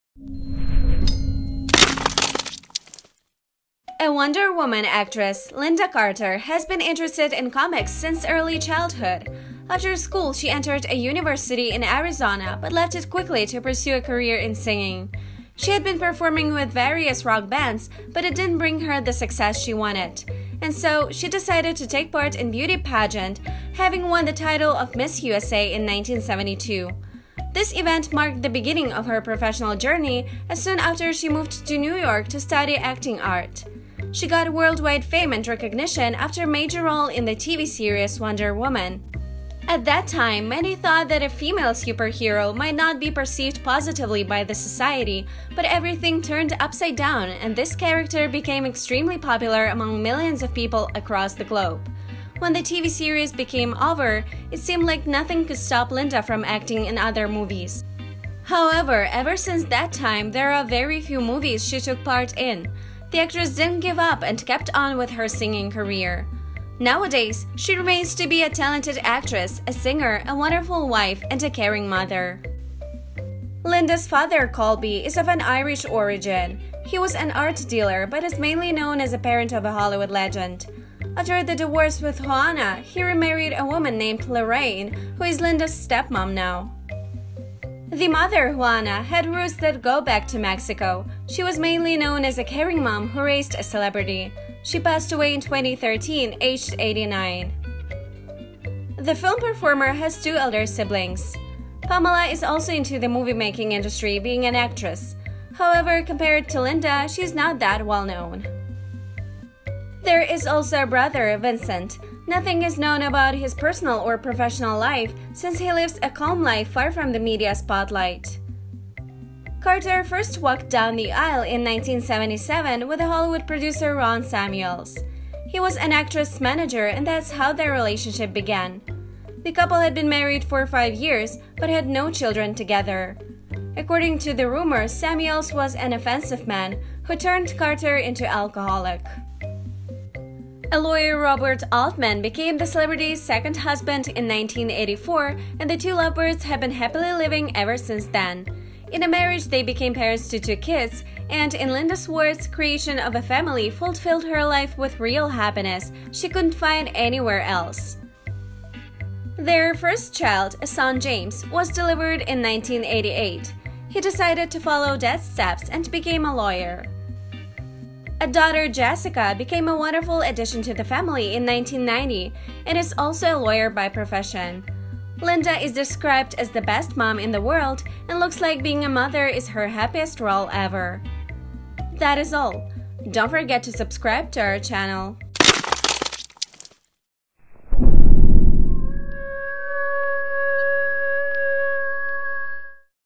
A Wonder Woman Lynda Carter's Family
A Wonder Woman actress Lynda Carter has been interested in comics since early childhood. After school, she entered a University in Arizona but left it quickly to pursue a career in singing. She had been performing with various rock-bands but it didn’t bring her the success she wanted. And so, she decided to take part in a beauty pageant, having won the title of Miss USA in 1972. This event marked the beginning of her professional journey as soon after she moved to New York to study acting art. She got worldwide fame and recognition after major role in the TV series Wonder Woman. At that time, many thought that a female superhero might not be perceived positively by the society, but everything turned upside down and this character became extremely popular among millions of people across the globe. When the TV series became over, it seemed like nothing could stop Lynda from acting in other movies. However, ever since that time, there are very few movies she took part in. The actress didn’t give up and kept on with her signing career. Nowadays, she remains to be a talented actress, a singer, a wonderful wife and a caring mother.

Lynda’s father Colby is of an Irish origin. He was an art dealer but is mainly known as the parent of a Hollywood legend. After the divorce with Juana, he remarried a woman named Larraine, who is Lynda’s stepmom now.

The mother, Juana had roots that go back to Mexico. She was mainly known as a caring mom, who raised a celebrity. She passed away in 2013, aged 89.

The film performer has two elder siblings. Pamela is also into the moviemaking industry being an actress. However, compared to Lynda, she is not that well-known.

There is also a brother Vincent. Nothing is known about his personal or professional life, since he lives a calm life far from the media spotlight.

Carter first walked down the aisle in 1977 with a Hollywood producer Ron Samuels. He was the actress’ manager and that’s how their relationship began. The couple had been married for five years but had no children together. According to the rumors, Samuels was an offensive man, who turned Carter into alcoholic.

A lawyer Robert Altman became the celebrity’s second husband in 1984 and the two love birds have been happily living ever since then. In a marriage they became parents to two kids and in Lynda’s words, creation of a family fulfilled her life with real happiness, she couldn’t find anywhere else.

Their first child, a son James was delivered in 1988. He decided to follow dad’s footsteps and became a lawyer.

A daughter Jessica became a wonderful addition to the family in 1990 and is also a lawyer by profession. Lynda is described as the best mom in the world and looks like being a mother is her happiest role ever.